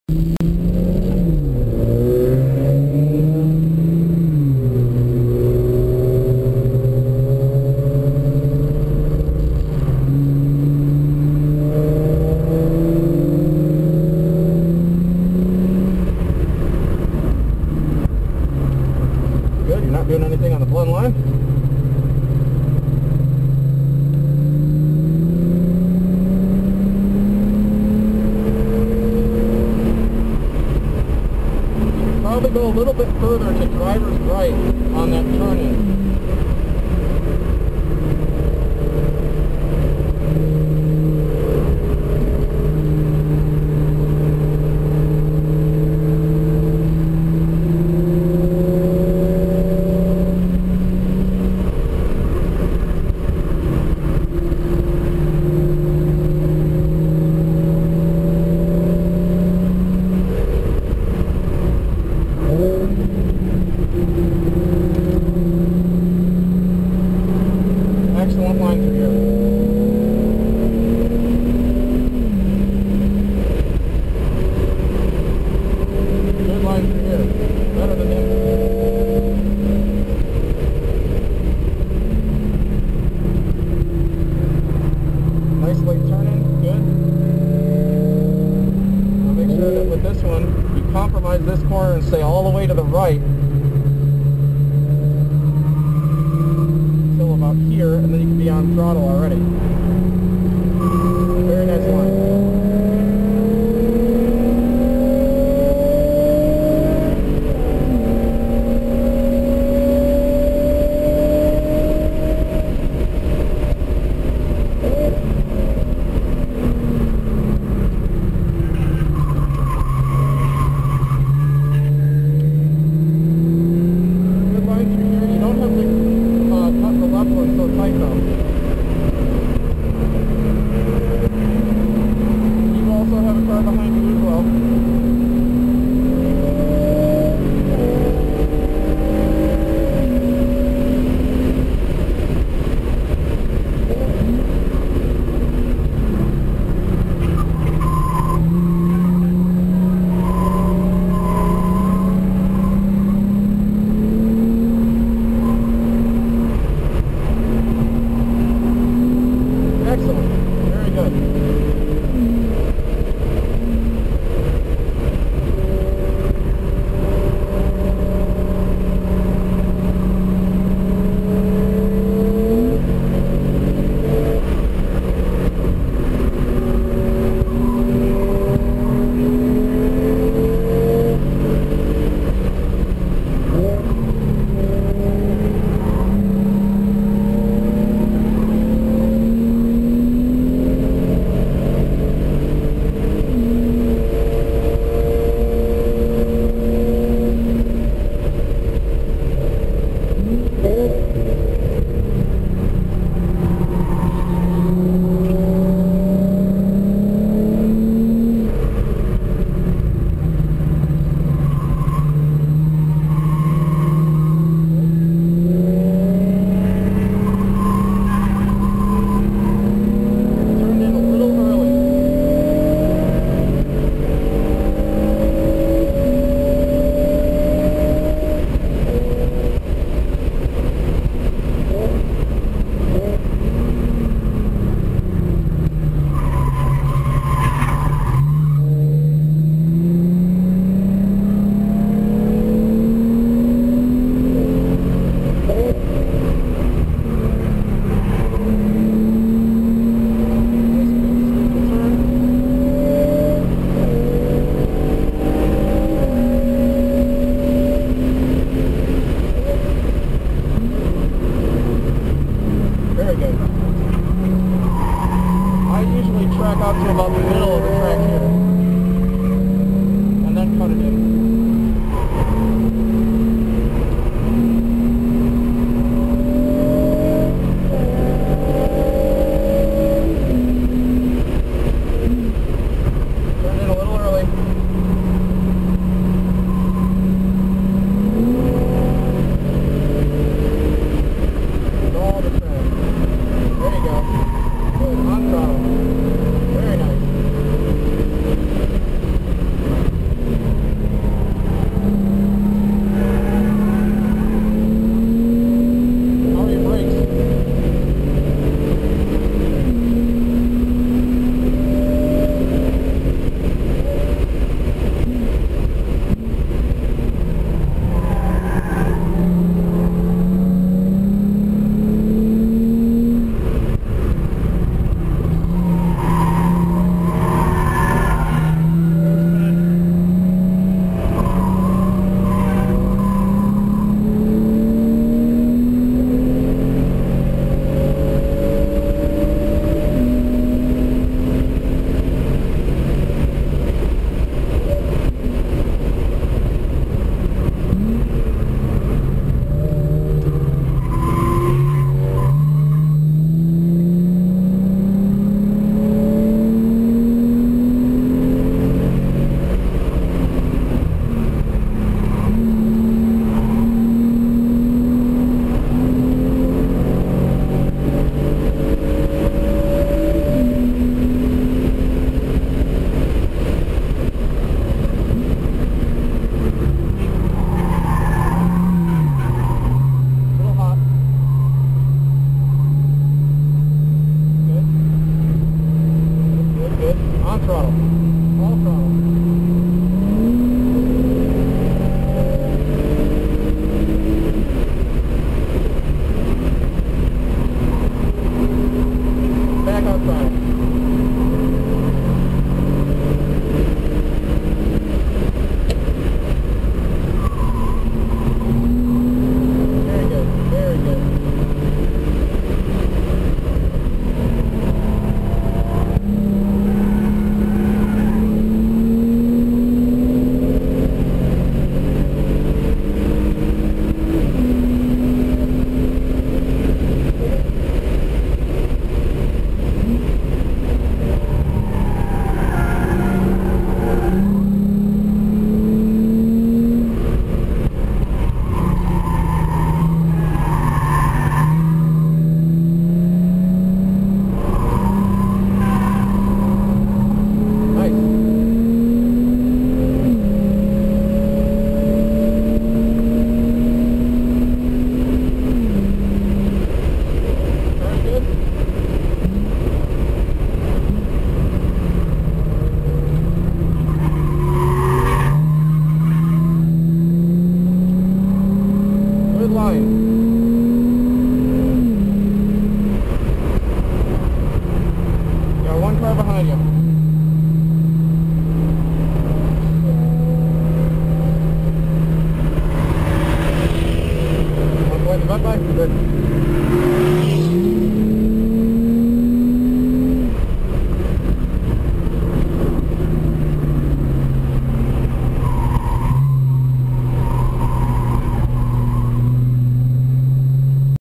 2012 Scca PDX 2 South Track in Genesis Coupe 3.8 (2nd session)
2nd PDX track day session #2 via SCCA Chicago region at Autobahn Country Club in Joliet, IL
another opportunity to let the Genesis Coupe 3.8 equipped with an ECU tuned by BTRcc stretch its legs on the South track
towards the end, the transmission went into limp mode due to high temperatures